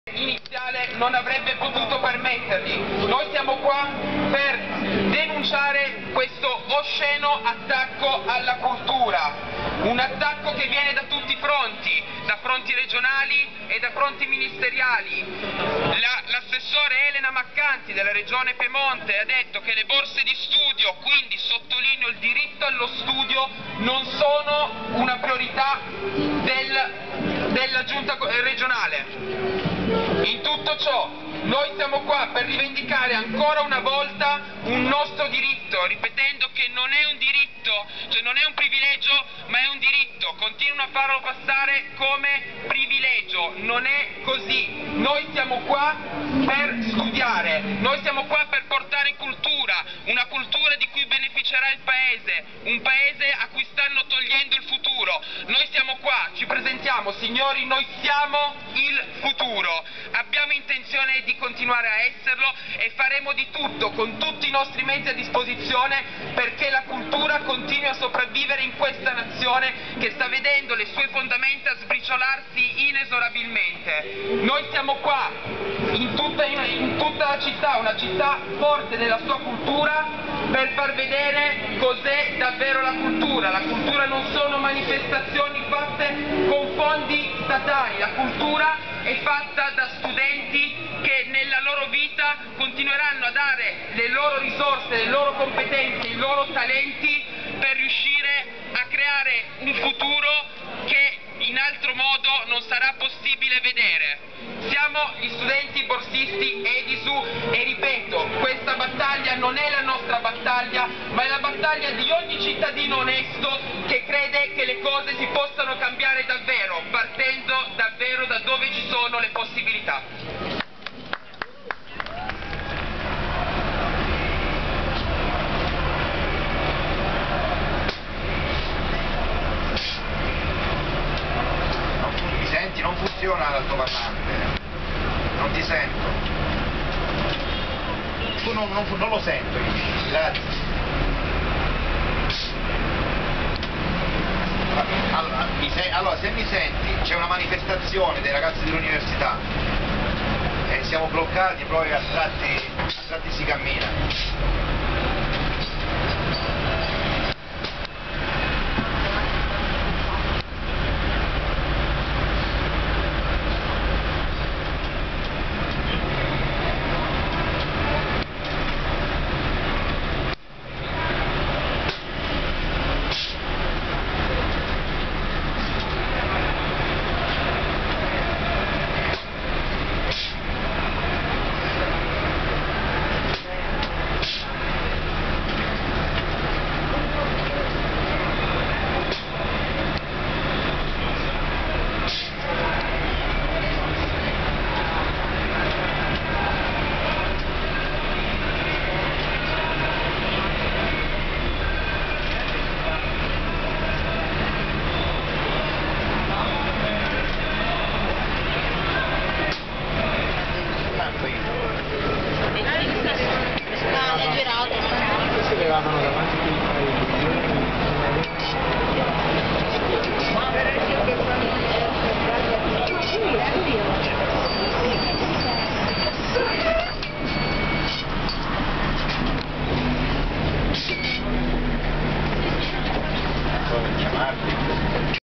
Manifestazione degli studenti EDISU
Manifestazione degli studenti EDISU a Torino il 27 novembre 2011. Piccoli frammenti filmati.